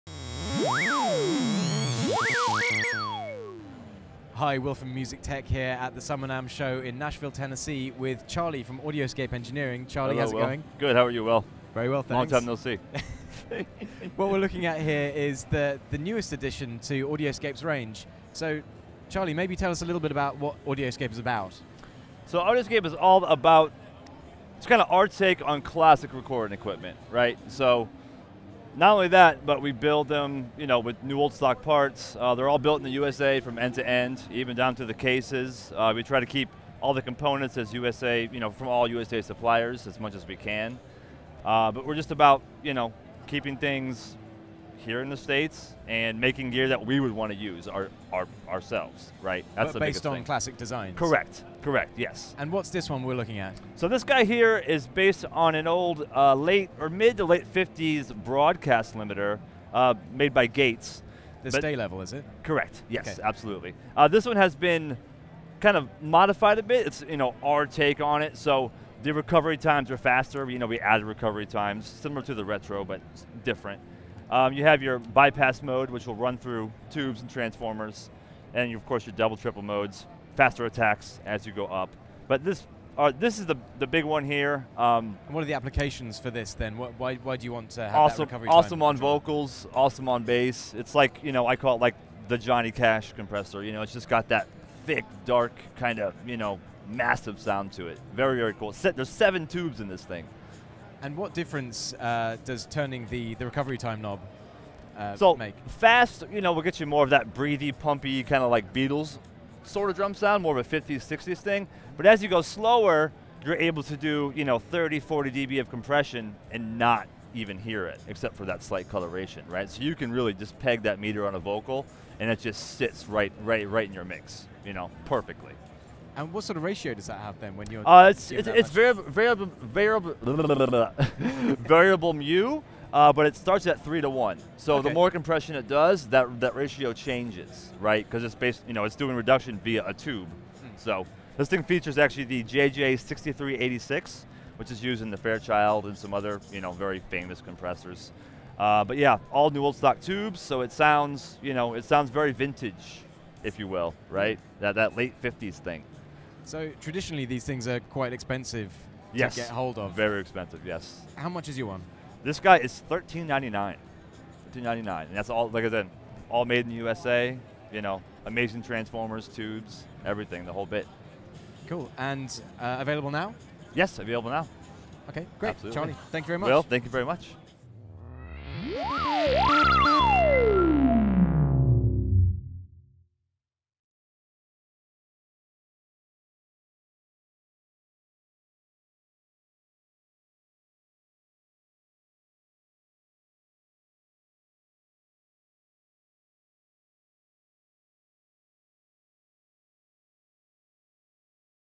Audioscape Engineering V-Comp recreates a classic vocal compressor
Audioscape Engineering has introduced the V-Comp, a vocal compressor that takes its cues from a certain Gates forebear. The unit gets its compression flavour from seven tubes, one of which is the JJ 6386 preamp tube. Learn more in the video above.